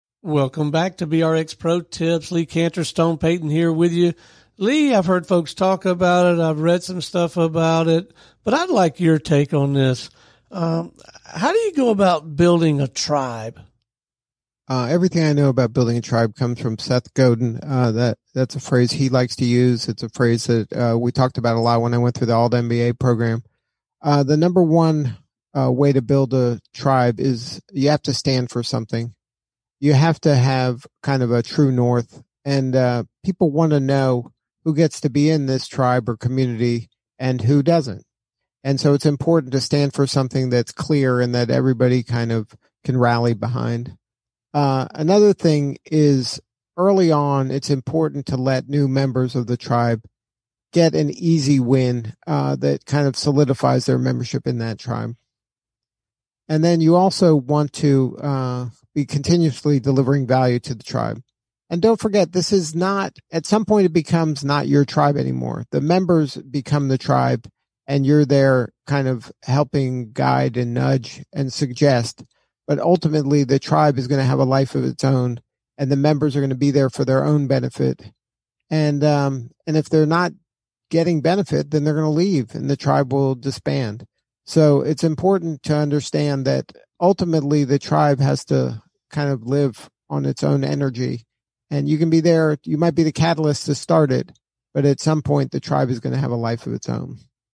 BRX Pro Tip: How to Build a Tribe
BRX Pro Tip: How to Build a Tribe Stone Payton: [00:00:00] Welcome back to BRX Pro Tips. Lee Kantor and Stone Payton here with you. Lee, I’ve heard folks talk about it. I’ve read some stuff about it. But I’d like your take on this. How do you go about building a tribe? Lee Kantor: […]
The post BRX Pro Tip: How to Build a Tribe appeared first on Business RadioX ®.